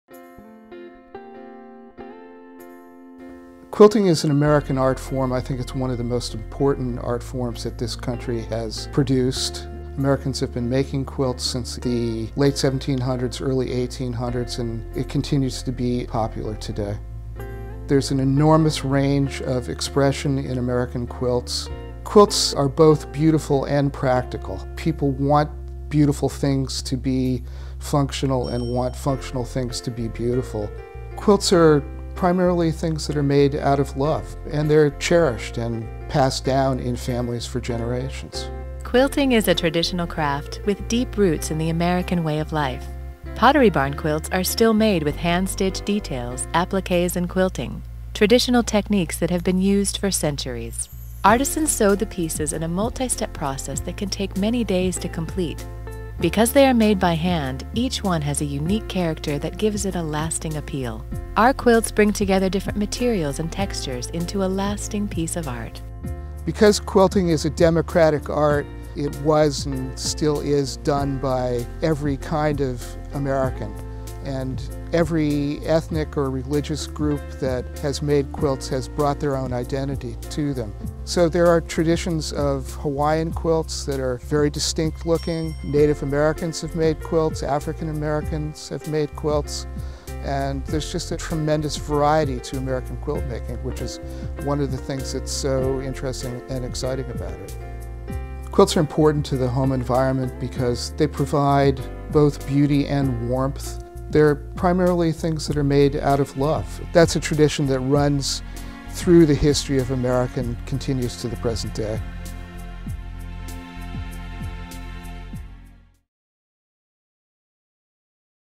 Traditional Quilts: An American Art | Pottery Barn
Quilt making is an old American art. The craft of quilt making dates back to more than three centuries and is reflective of the varied cultural and demographic interpretations of art within the country. In this video, Antique Quilt Expert Robert Shawn explains the tradition of American quilting in detail.

According to Shawn, the art of quilt making in America has deep roots in the American way of life. The traditional quilts are still made with the help of handmade techniques. Pottery Barn has a huge collection of handmade quilts which bring together different materials and textures.

Shawn says that the diversity of quilting designs is due to the vast ethnic diversity that is involved in the art of quilt making in America. Pottery Barn has some of the most exotic collections of traditional quilts in its list as a tribute to this rich American art.